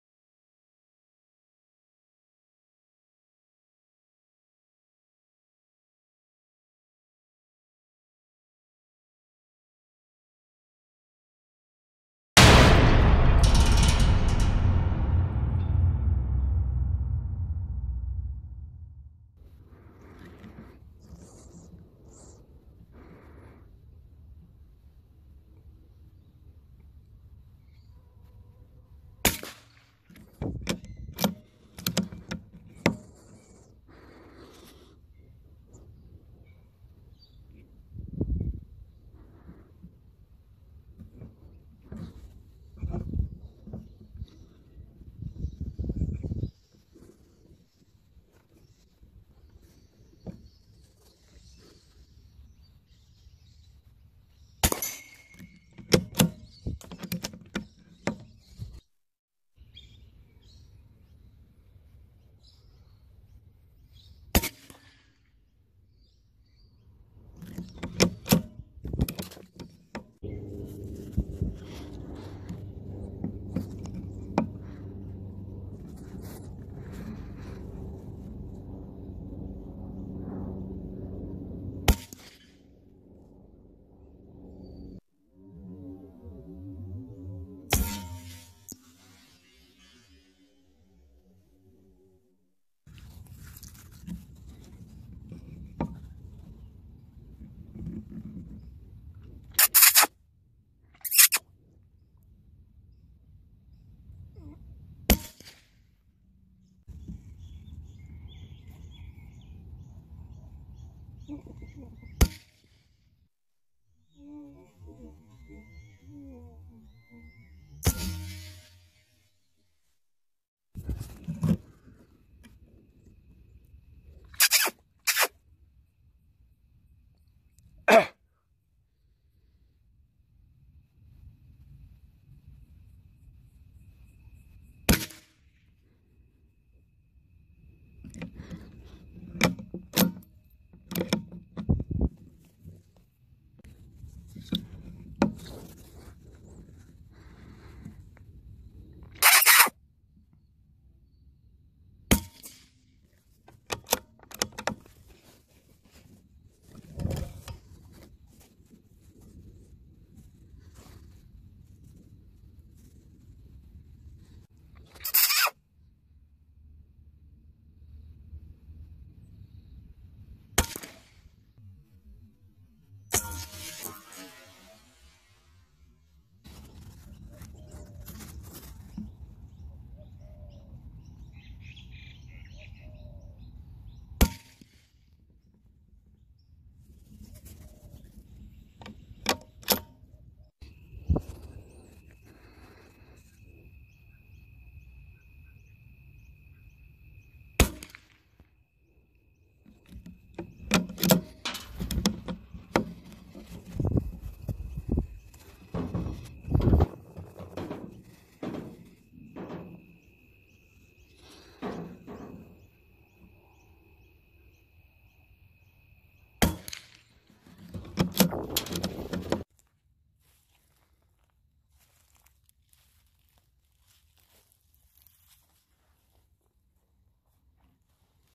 Rabbit hunting/pest control with a .22lr CZ452 rimfire in Australia -11 rabbits shot within 1/2hr.
I had a job around the corner from this property that I hunt on, and decided to bring the rifle with me to work. just in case I finished work early enough to squeeze a hunt in before it got dark.
Well, I had about 1 hour before it got dark, so I set up my new camera mount and was left with only 1/2 hr of day light left to spare, so this is all I could fit in before it got dark.
I ended up with 11 rabbits in 30 minutes.
Hopefully I get an opportunity to hunt on a bigger property next time,  that way I get to record some of the longer ranged shots.
I hope you enjoy it! Cheers.

Rifle is a CZ452 American.
Scope is a Leupold VXII 6-18x40.
Ammunition I use is the Winchester 42 grain subsonics.
Camera is my galaxy S9 phone with the Side Shot Scope Phone Mount.
The rifle is sighted in at 60 metres, hence why I aim a little lower on the ones at closer range.